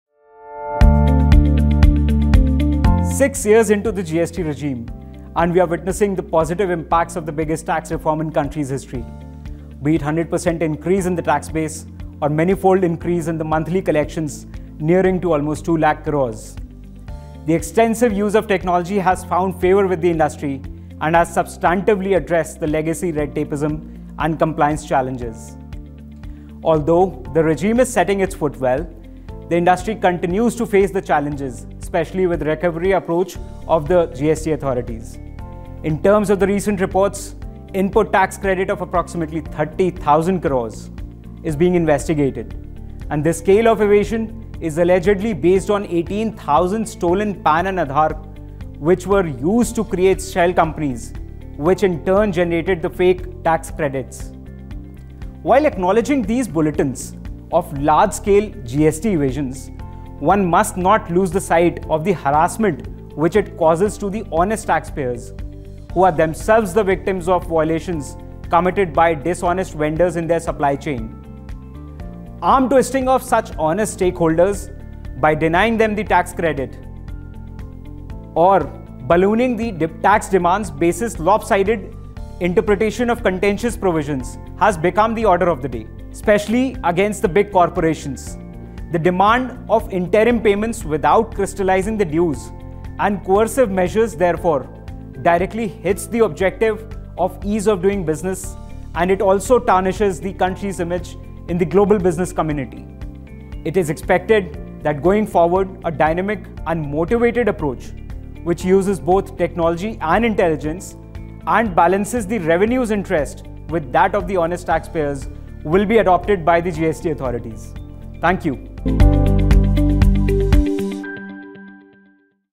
6 years of GST – Ayush Mehrotra on Impact and Challenges of the GST Reform in India
The Goods & Services Tax (GST) has made a tremendous contribution to the Indian economy, with the number of taxpayers having doubled and the average monthly revenue numbers rising significantly. Ayush Mehrotra, Partner speaks on about his expectations vis-a-vis suitable checks and balances to be introduced in the ongoing enforcement actions by the GST authorities against GST input credit frauds.